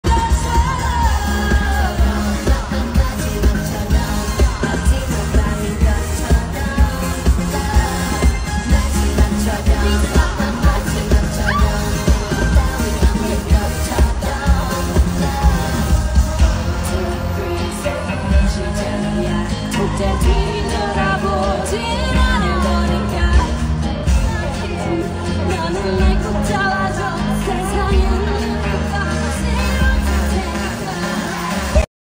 Rosé pull Lisa back to stage | 唱得太開心的 Lisa | BLACKPINK 《BORN PINK》 in Kaohsiung
Credit: ooo_lin_ooo (Instagram)